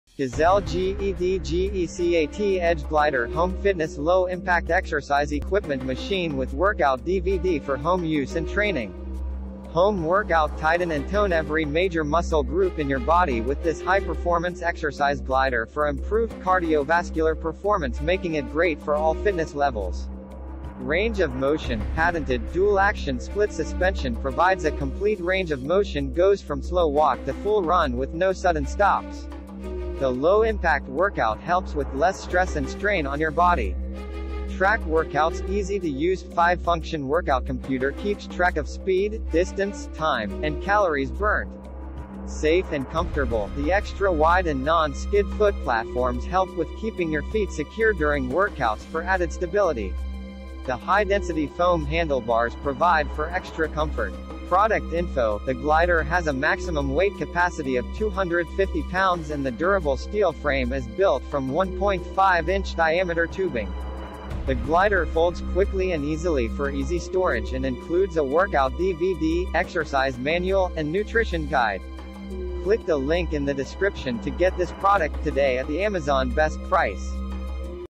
Gazelle GEDGECAT Edge Glider Home Fitness Low Impact Exercise Equipment Machine with Workout DVD
Click here for the best price:

Gazelle GEDGECAT Edge Glider Home Fitness Low Impact Exercise Equipment Machine with Workout DVD for Home Use and Training

HOME WORKOUT: Tighten and tone every major muscle group in your body with this high-performance exercise glider for improved cardiovascular performance making it great for all fitness levels
RANGE OF MOTION: Patented, dual-action split suspension provides a complete range of motion goes from slow walk to full run with no sudden stops. The low impact workout helps with less stress and strain on your body
TRACK WORKOUTS: Easy-to-use, 5-function workout computer keeps track of speed, distance, time, and calories burnt
SAFE & COMFORTABLE: The extra-wide and non-skid foot platforms help with keeping your feet secure during workouts for added stability. The high-density foam handlebars provide for extra comfort
PRODUCT INFO: The glider has a maximum weight capacity of 250 pounds and the durable steel frame is built from 1.5-inch diameter tubing. The glider folds quickly and easily for easy storage and includes a workout DVD, exercise manual, and nutrition guide